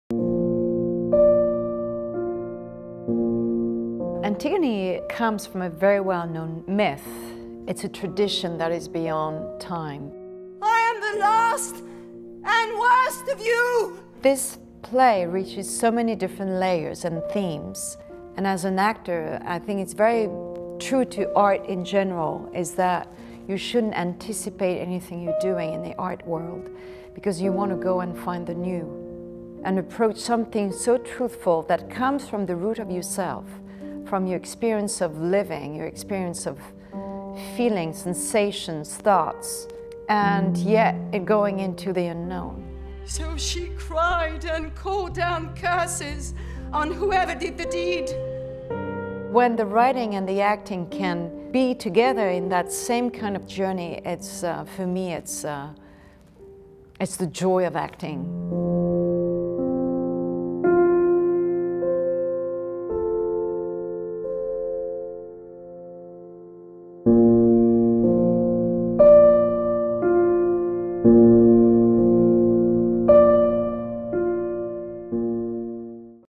Behind the Curtain: Antigone with Juliette Binoche
Academy Award–winning stage-and-screen actress Juliette Binoche discusses her portrayal of Antigone in a contemporary version of Sophokles's riveting tragedy, translated anew by T.S. Eliot Prize–winning poet Anne Carson.